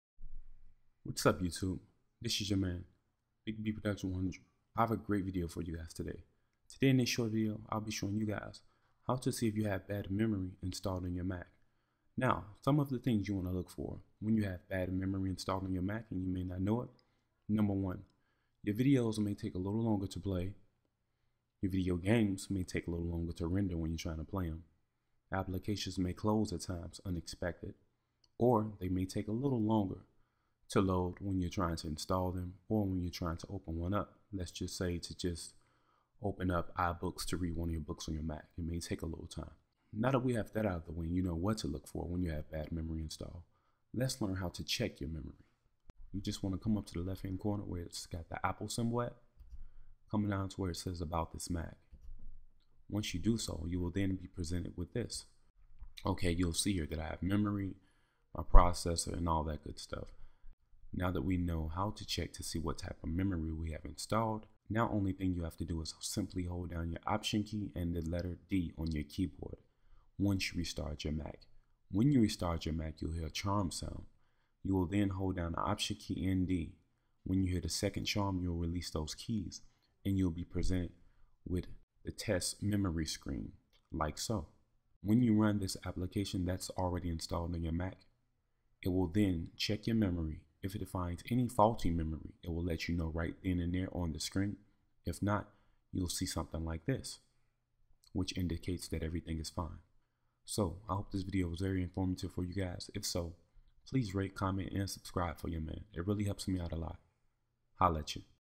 How To Test Your Mac For Bad Ram On OS X
RAM in your Mac is faulty, those faults can sometimes persist undetected for a while, only to crop up unexpectedly and result in a crash, hang, or other unwanted behavior. Therefore, it is good to not only be able to identify faulty RAM, but also be able to properly test for it and then be prepared to fix the problem. So In this video I'll show you how to see If yours needs a fixing.